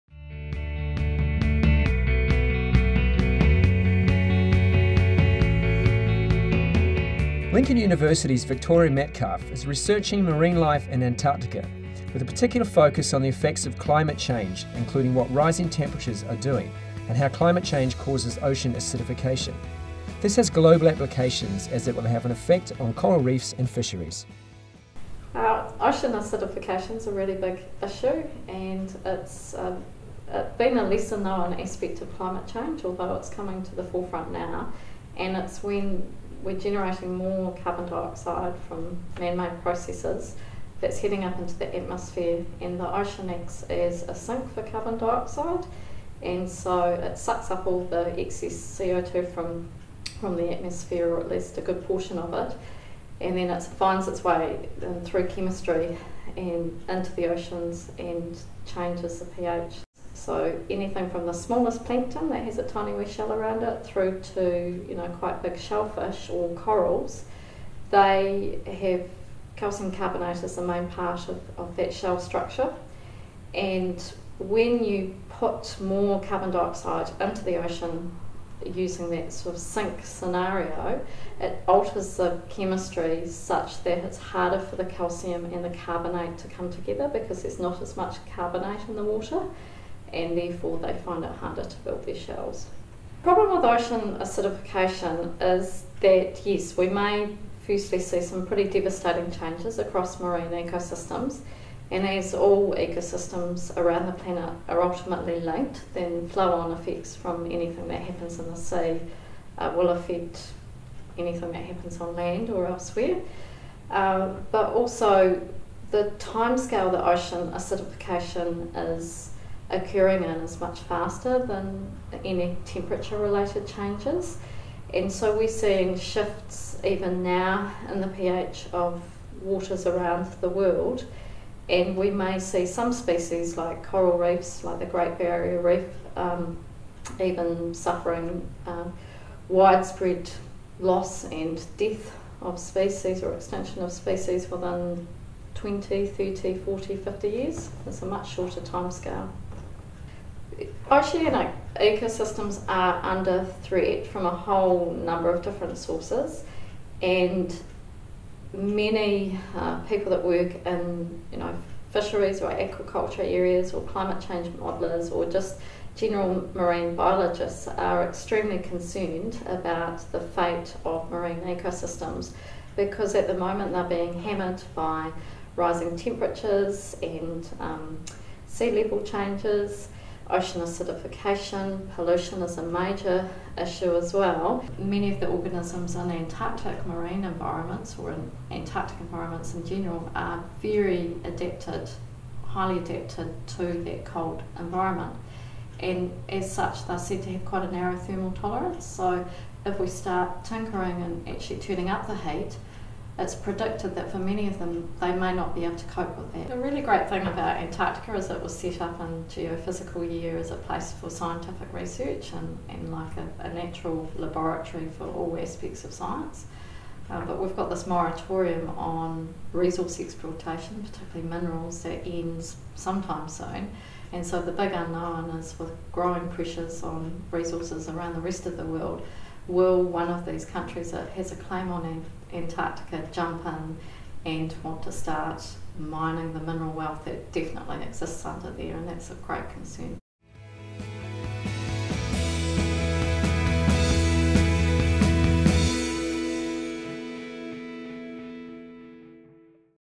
Antarctic fisheries
Dr Victoria Metcalf, lecturer in animal genetics, discusses her research on Antarctic marine life and climate change.